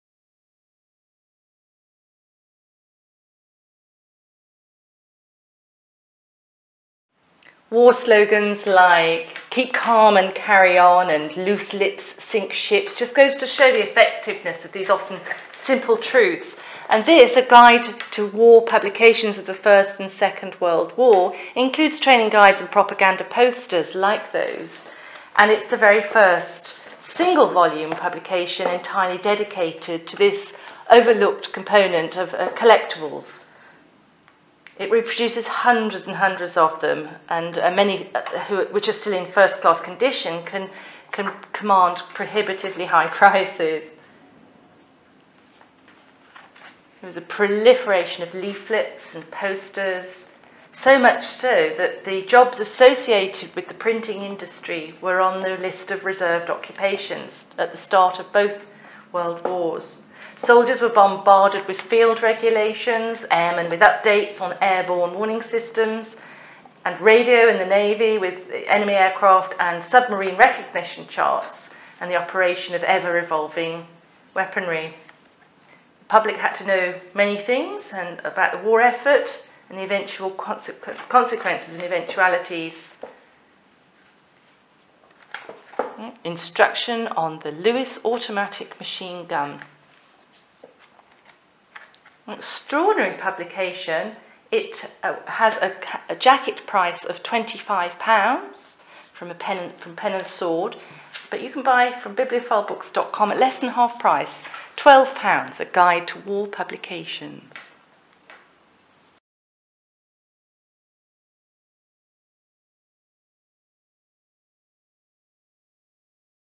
GUIDE TO WAR PUBLICATIONS OF THE FIRST AND SECOND WORLD WAR
ARTHUR WARD    Book Number: 83010    Product format: Hardback

In both world wars there was a proliferation of leaflets and posters, so much so that most of the jobs associated with the printing industry were on the list of reserved occupations, at least at the start of both wars. Soldiers were bombarded with Field Regulations, airmen with updates on airborne warning systems and radio and the navy with enemy aircraft and submarine recognition charts and the operation of ever-evolving weaponry. The public on all sides were subject to even more: how best to help the war effort, what to do in all eventualities, the consequences of irresponsible behaviour and propaganda in all its many forms. The popularity today of variants of the "Keep Calm and Carry On" classic and "Loose Lips Sink Ships" just goes to show the effectiveness of these often simple truths. This book is the first single volume publication dedicated entirely to this long overlooked component of wartime collectibles. Not only does it reproduce hundreds of them in their original colour or b/w but it also reveals how much of this material was produced, when and why, by all combatant nations. The posters in particular still often make attractive items for framing and display and much of this material can still be bought and sold quite easily - it is only that which is in first class condition that attracts prohibitively high prices. A separate appendix examines the personality cult that surrounded Hitler and how it was eventually used against him, and another features Penguin special wartime publications. 204pp, colour and b/w illus.